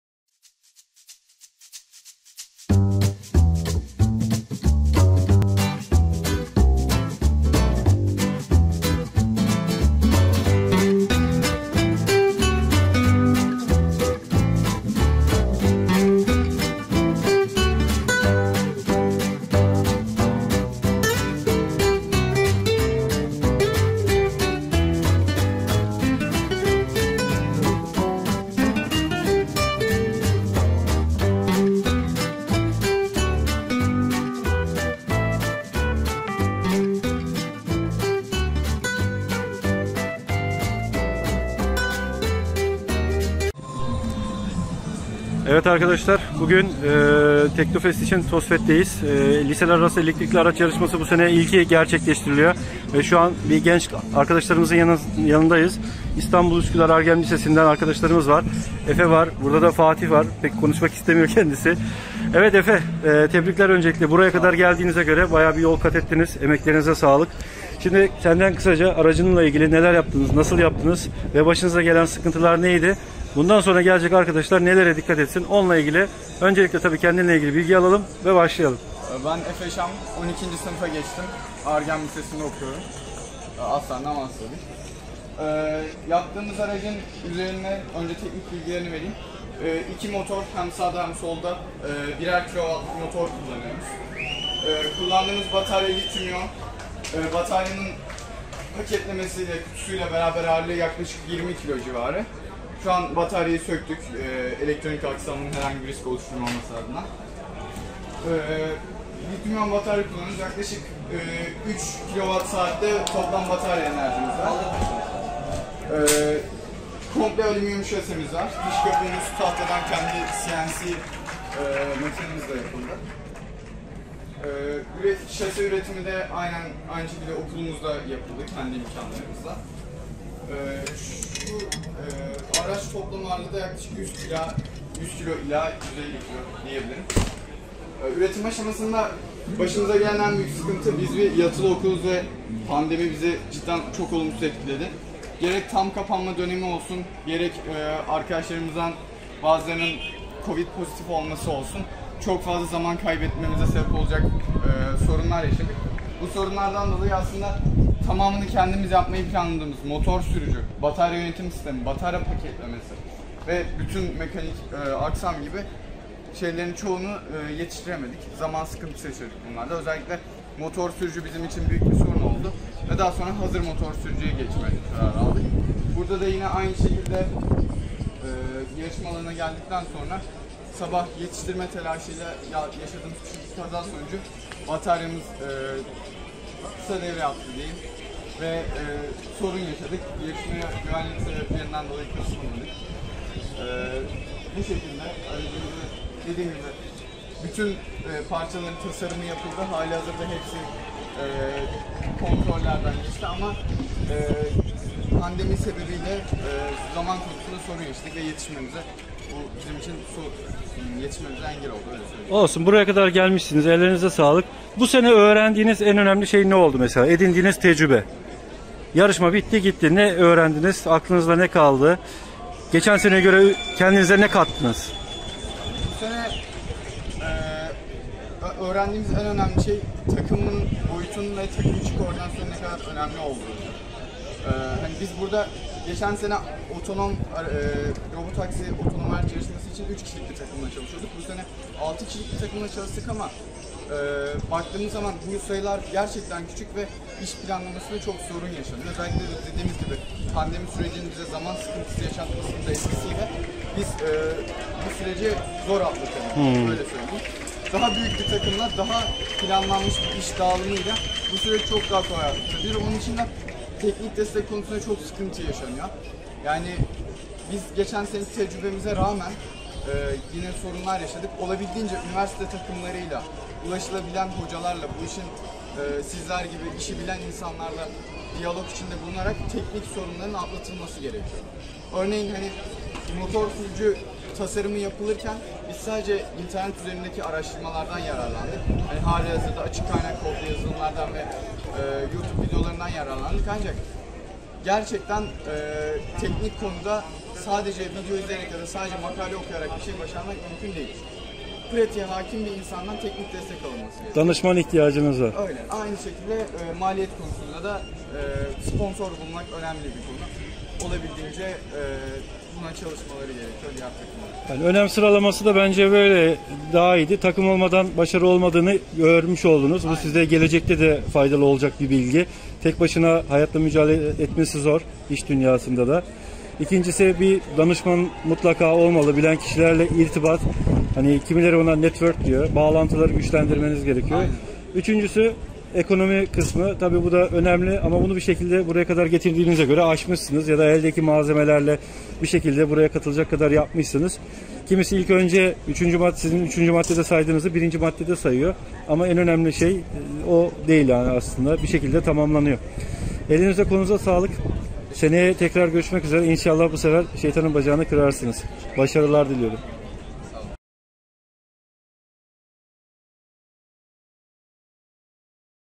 Üsküdar ARGEM Lisesi Elektrikli Aracı "AROTO"yu inceliyoruz ve tecrübelerini dinliyoruz.
Hazırlayan: Ümit YAVUZARSLAN // elekTRofizikci.
Herkese merhaba arkadaşlar. Geçen hafta TEKNOFEST ile birleştirilen Tübitak Elektrikli Araçlar Yarışması , her yıl olduğu gibi Körfez Yarış Pistinde gerçekleşti. Yarışmaların geçmişi 2005 yılına dayansa da milli teknoloji hamlesi kapsamında, TEKNOFEST etkinliği ile birleşince popülerliği daha da arttı. Tübitak bu yarışma ile de diğer yarışmalarda olduğu gibi alanında yetişmiş insan gücü yetiştirmeye çalışıyor. Bu yarışmalardaki ekipler, ilgili firmalarca takip edilip, kadrolarına dahil ediliyorlar veya öğrenciler bu alanda kendi start-up larını kuruyorlar. Örneğin, Kıbrıs'ın yerli araba markası GÜNSEL, ekibini bu yarışmalardaki öğrencilerden oluşturmuştur.
Bu videoda, bu yıl ilk defa liselerinde katılımıyla gerçekleşen etkinlikte bulunan, katılan ve benimde yakından takip ettiğim Araştırma Geliştirme Eğitim ve Uygulama Merkezi Lisesi (ARGEM) öğrencilerinin el emeği göz nuru aracı AROTO'yu inceledik. Gençlerin çektiği sıkıntıları ve tavsiyelerini dinledik.
Böyle birkaç röportaj daha sizlerle olacak.
Görüşmek üzere, hoşçakalın!